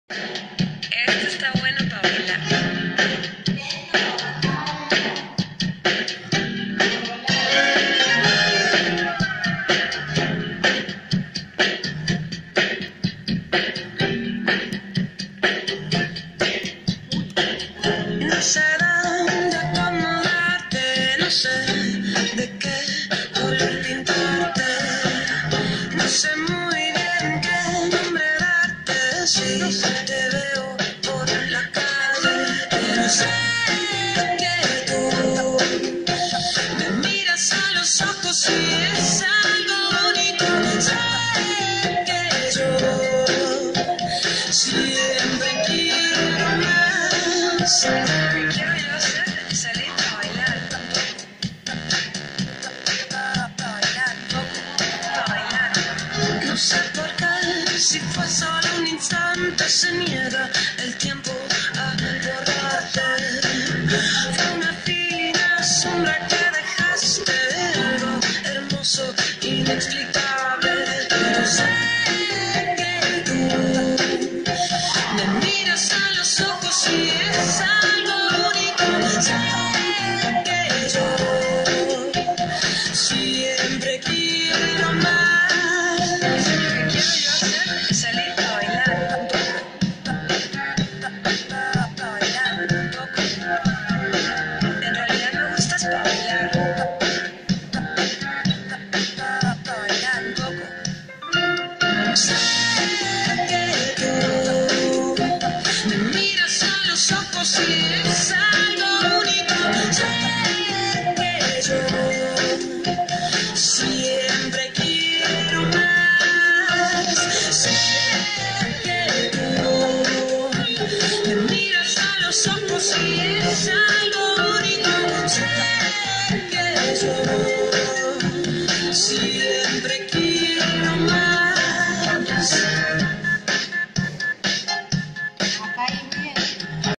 We love el suelo
Clases 2019 @rocio.corraless
Academia Integral de la Danza Santa Fe de la Prof. Mónica Oller
3 de febrero 3259 - Centro Piamontés
Bailan: Brisa Vallejos, Ludmila Grillo, Paula Portillo, Lucila Geneyro y Celina Castagnino